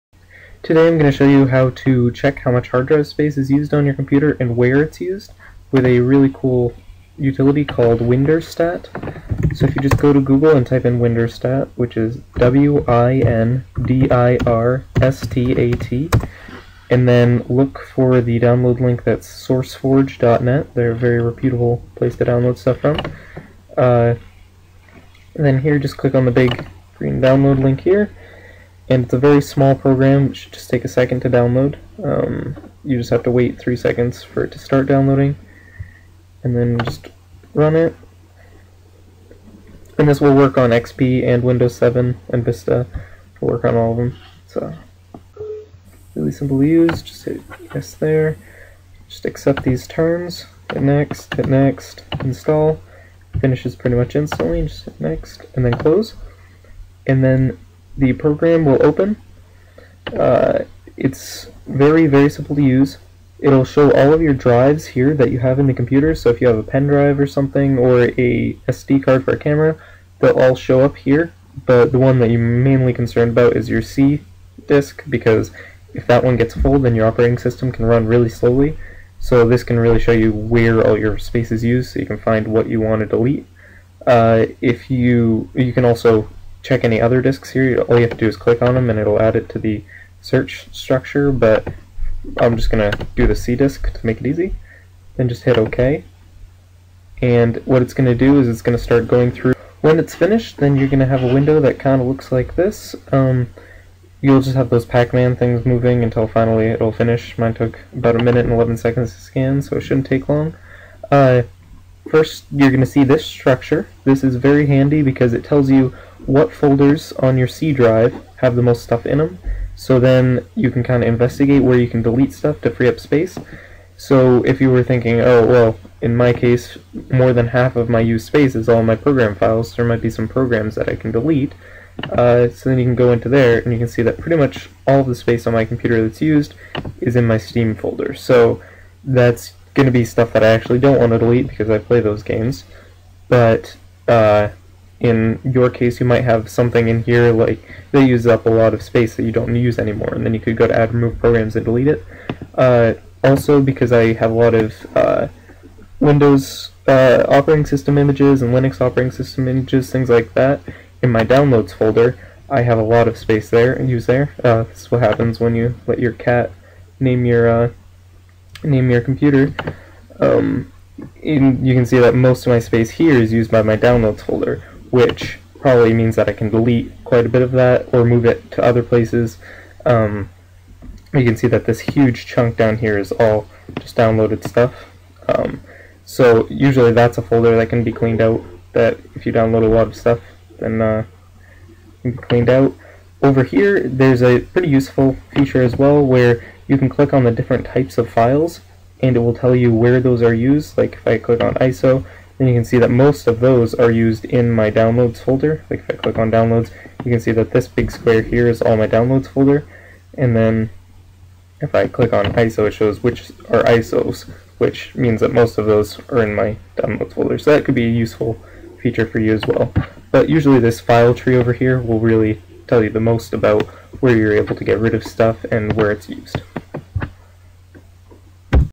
How to find where space is being taken up on your computer
This is a quick tutorial on how to use windirstat (a free program) to find which files and folders are using space on your coomputer's hard drive. This works in any windows operatings system and is very easy to use. This can be used to free up space on your computer when it is getting slowed down if it's full. This can also be used to find where large files are so you can move them to an external hard drive or to another drive on your computer.